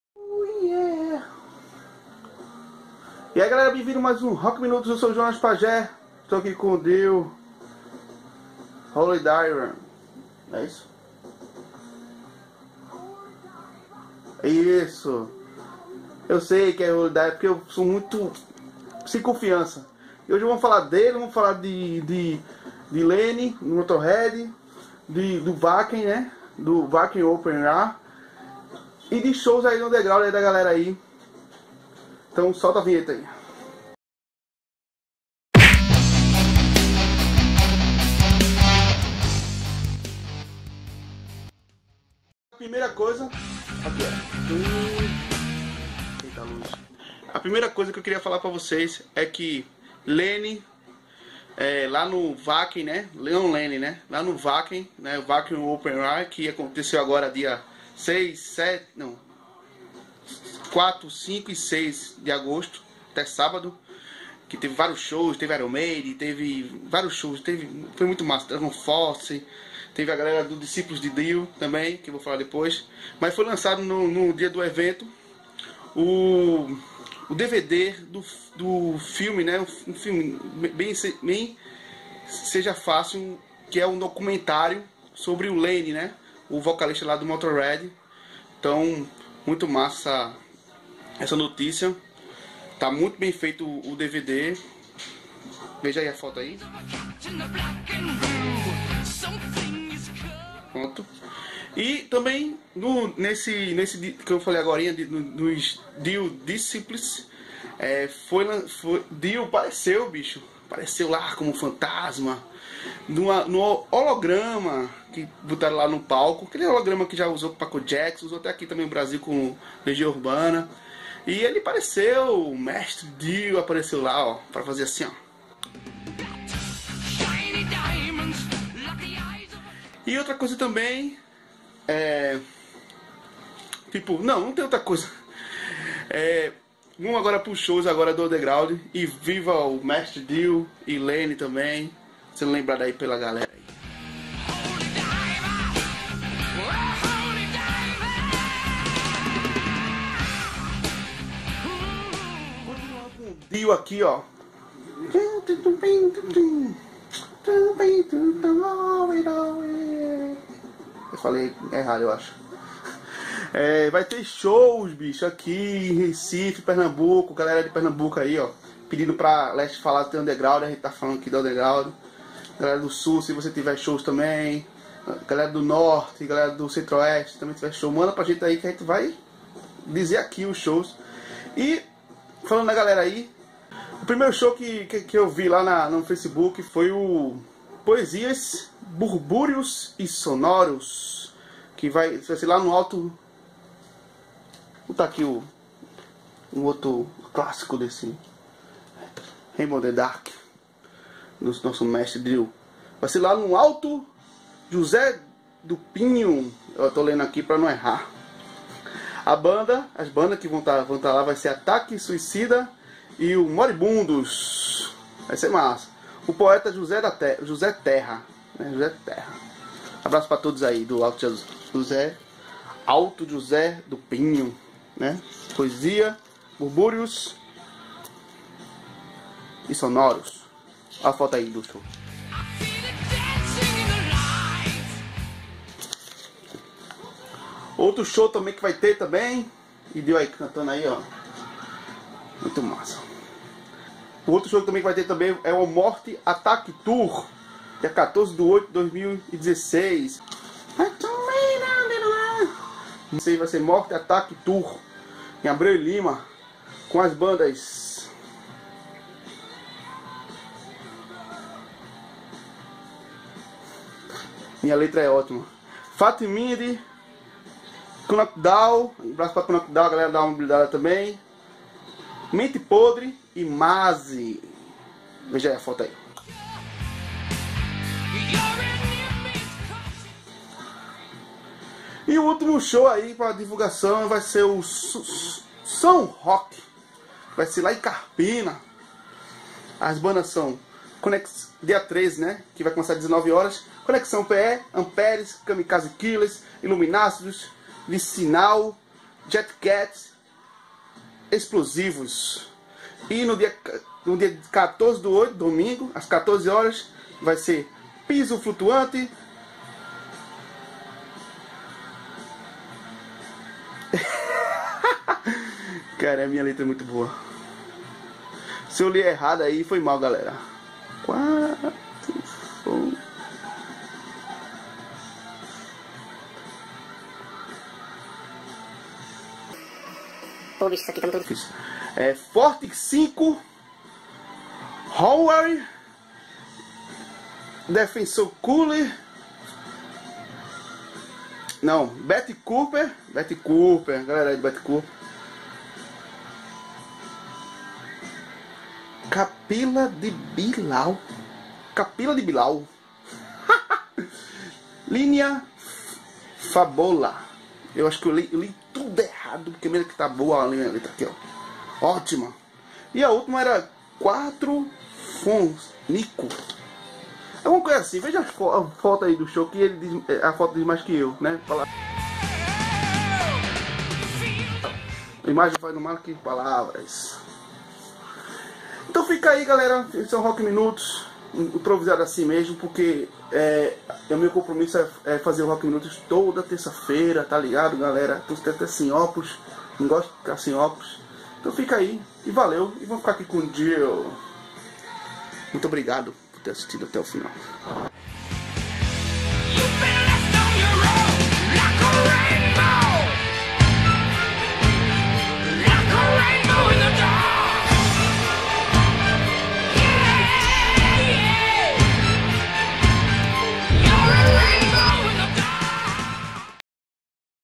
Lemmy The Movie, Holograma de Dio e Shows das bandas independentes de PE | Rock não Acabou #28.2
Hoje vamos noticiar os duas ocorrências iradas que aconteceu Wacken Open Air, o lancamento do Lemmy The Movie e o Holograma do mestre Ronnie James Dio, além disso vamos divulgar os Shows das bandas independentes de Pernambuco, segue detalhes das notícias de dos shows:

- Shows da Cena Underground Pernambucana -

Poesis Burbúrius Sonorus - Bandas: Ataque Suicida e Moribundos, Poeta, José Terra. Data 12/08/2016 as 19h, Rua Vespasiano, 107, Alto José do Pinho, Recife-PE.

NORTE ATTACK TOUR 3 - Bandas: JOHNNY 13 (Esqueci essa, Desculpa!),KNOC DOWN, MAZZE HC, FREE MIND, JEEZ COCK (Esqueci essa também, Desculpa!) e MENTE PODRE. Dia: 14/08/2016, Local: Praça São José (Abreu e Lima Centro), Horário: 15h.

São Rock - Bandas: SÁBADO - 13 de agosto 2016 as 19h, Iluminacidos, Kamikaze killers, Conecxão PE, Amperes, Jet Cats e Viccinal, Perguntei ao Evento sobre a banda Explosíveis kkkkkkkkkk.. Se não tiver é pq que li a mais no folder do evento. kkkkkkk.. Mas segue então uma sugestão pra nomes de bandas.
São Rock - Bandas: Domingo - 14 de agosto 2016 as14h, Howay Forte 5, Quadrafonico, Piso Flutuante, Bettercup, Capa de Bilau e Lyllian Tabosa.  Local Rua Rosita Freire, 223, Carpina - PE.

VALEUUUUU!!
Abraços a todos!
Viva o Rock!!!